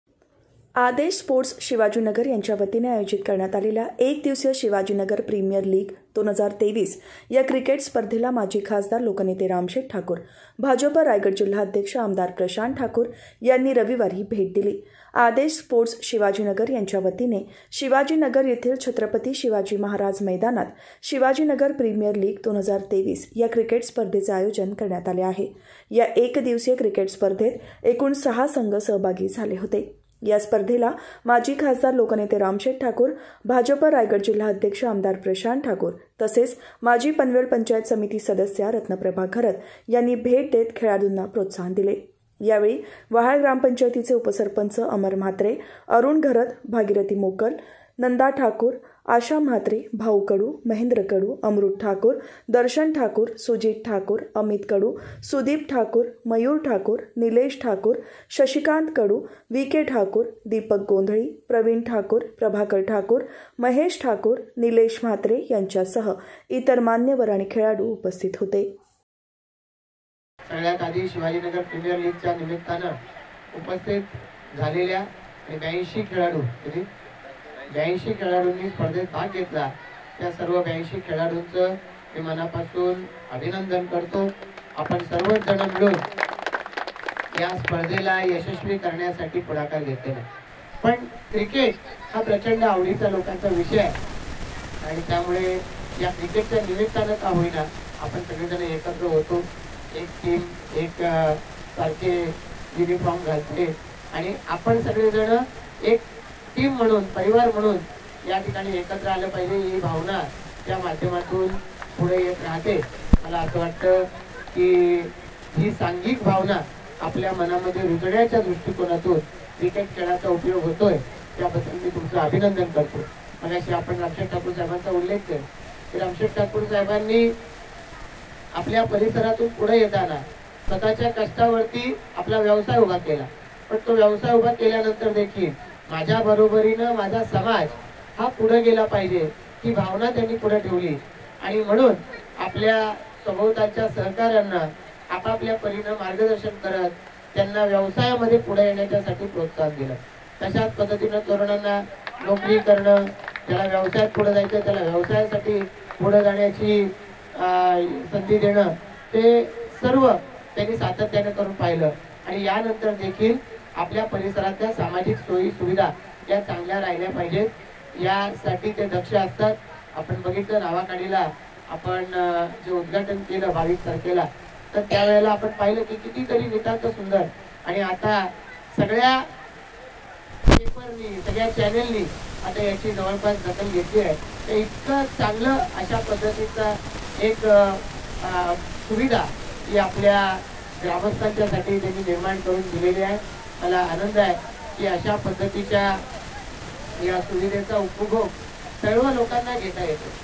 शिवाजीनगर प्रीमियर लीग रंगली; लोकनेते रामशेठ ठाकूर, आमदार प्रशांत ठाकूर यांची प्रमुख उपस्थिती !
आदेश स्पोर्ट्स शिवाजीनगरच्या वतीने रविवारी (दि. 1) आयोजित एकदिवसीय शिवाजीनगर प्रीमियर लीग ही क्रिकेट स्पर्धा उत्साहात झाली. या स्पर्धेला माजी खासदार लोकनेते रामशेठ ठाकूर, भाजप रायगड जिल्हा अध्यक्ष आमदार प्रशांत ठाकूर यांनी भेट देत खेळाडूंना प्रोत्साहन दिले.